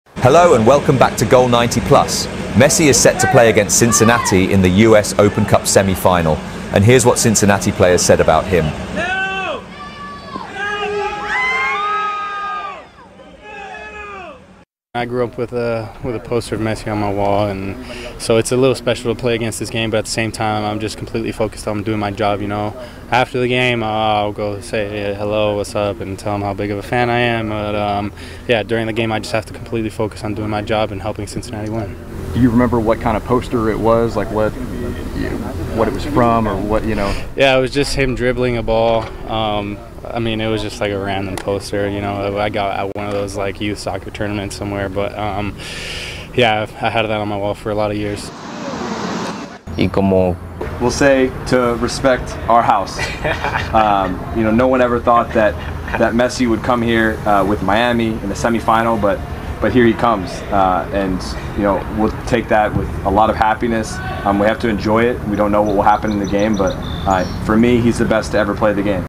Cincinnati players reaction to Messi vs Cincinnati in US Open Cup semifinal
Messi news today. Messi vs Cincinnati. Inter Miami vs Cincinnati US Open Cup semi final